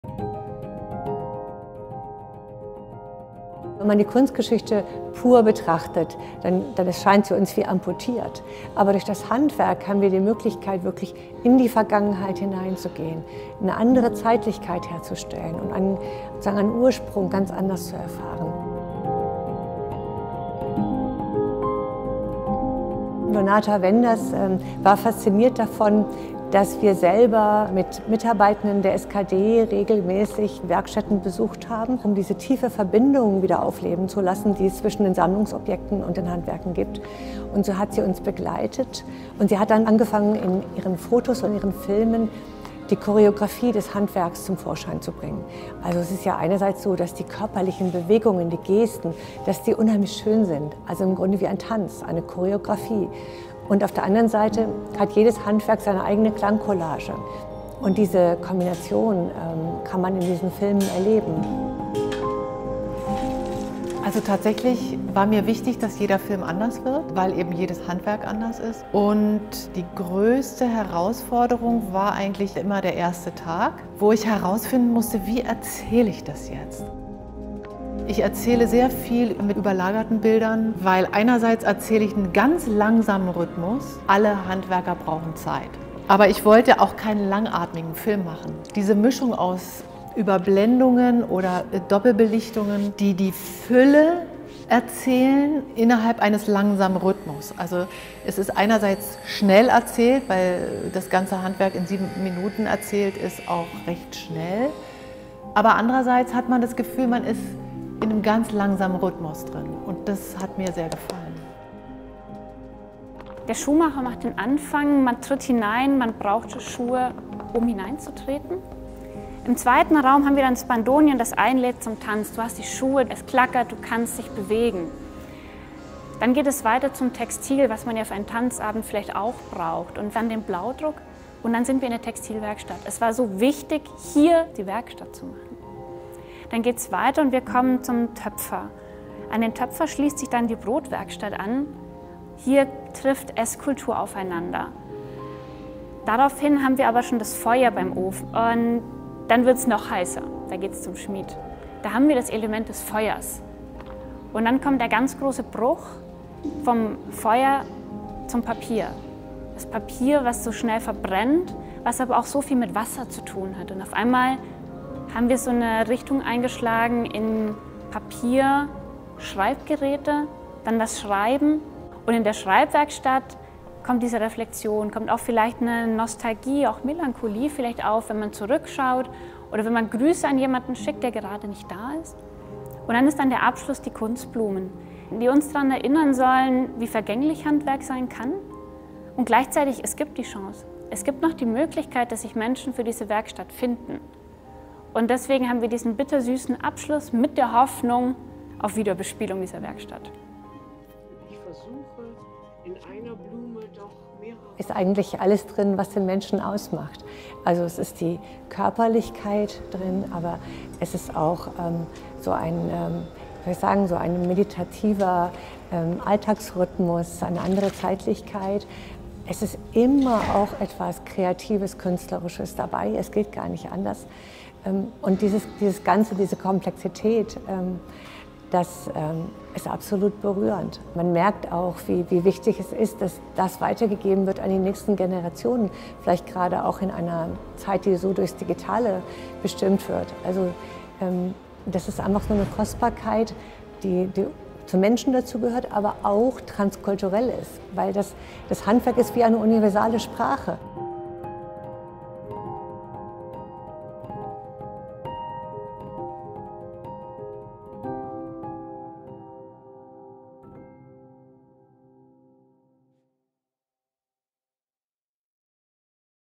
Ode an das Handwerk mit Filmen von Donata Wenders | Japanisches Palais (24.11.2023—07.04.2024)
Die Sonderausstellung „Ode an das Handwerk“ mit Filmen von Donata Wenders aus den Jahren 2019 bis 2023 porträtiert Handwerk in Sachsen. In zehn Leinwandprojektionen werden Gewerke in den Fokus genommen, die charakteristisch für diese traditionsreiche Region sind und die mitunter vom Verschwinden bedroht sind, darunter die Kunstblumenherstellung.